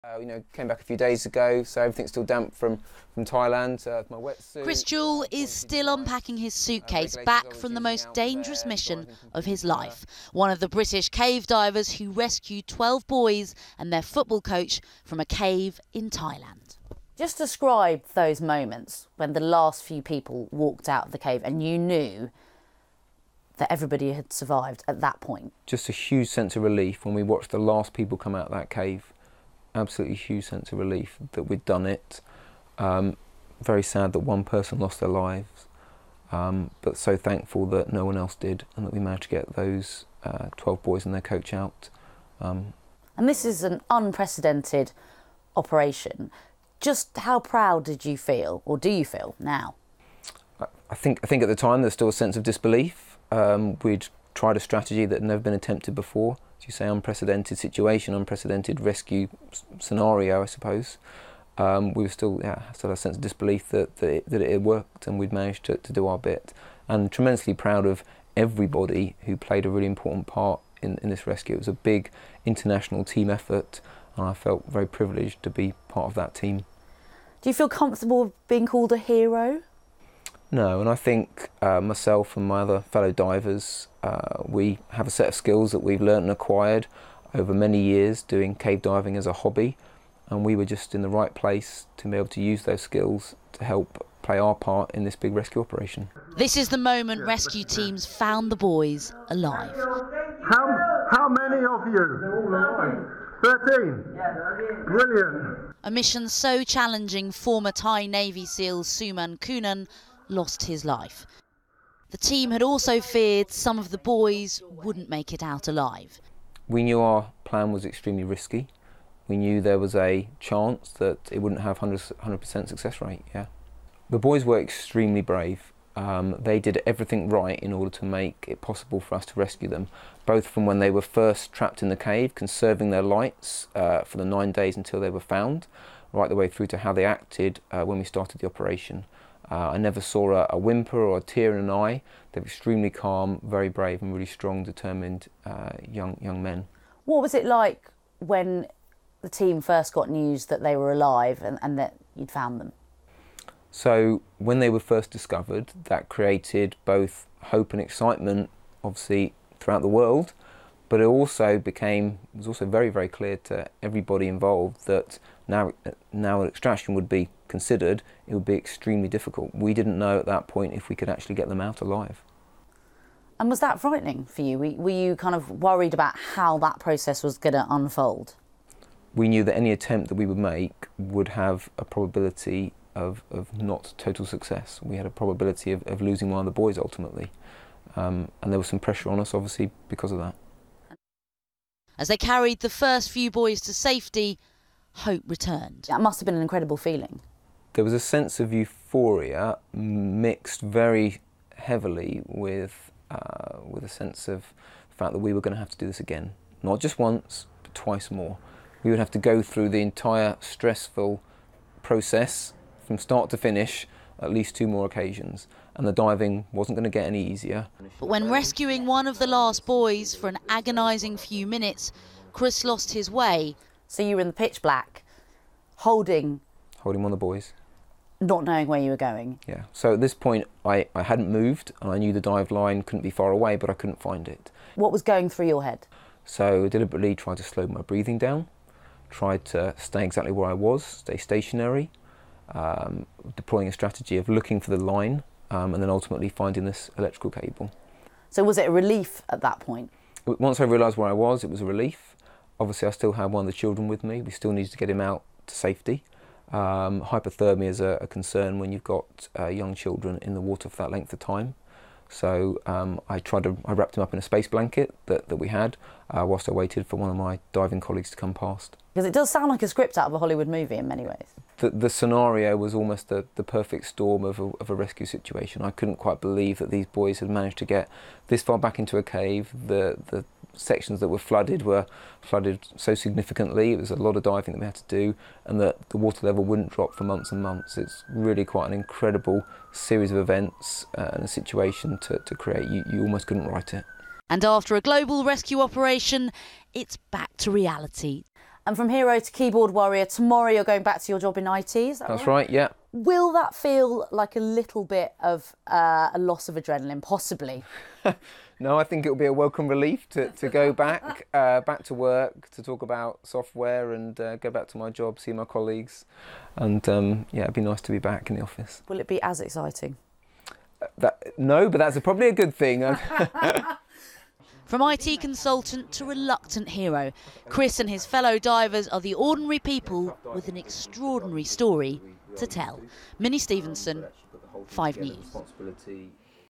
Thai cave rescue: British diver lost rope guide for four minutes during mission | 5 News
► One of the British divers involved in the extraordinary rescue of 12 boys from a cave in Thailand has been telling 5 News how it nearly went badly wrong.

He says at one point, while guiding one of the boys, he lost his grip for nearly 4 minutes - and feared they would never get out.